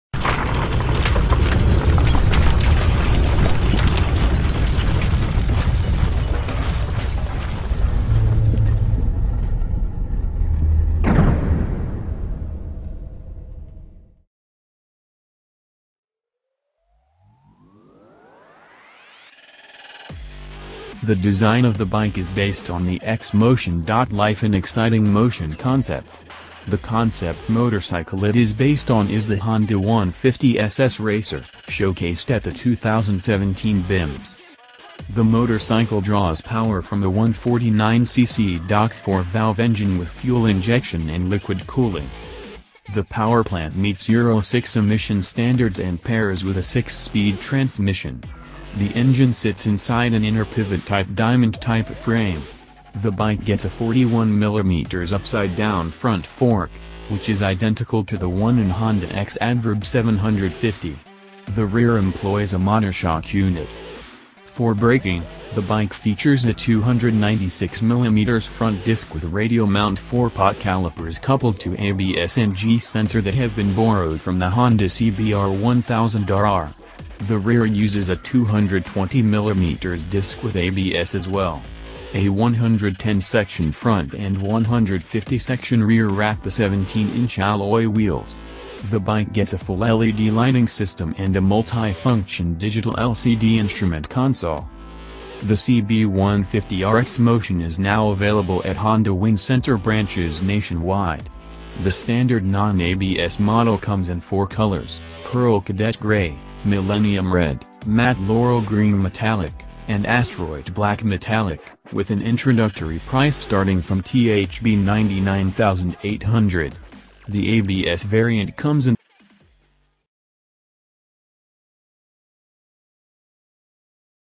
MUST WATCH !!!!2018 Honda CB150R ExMotion Price & Spec
MUST WATCH !2018 Honda CB150R ExMotion Price & Spec - The design of the bike is based on the ‘ExMotion…Life in Exciting Motion’ concept. The concept motorcycle it is based on is the Honda 150SS Racer, showcased at the 2017 BIMS.

██████ GET DAILY UPDATES! NOW! ██████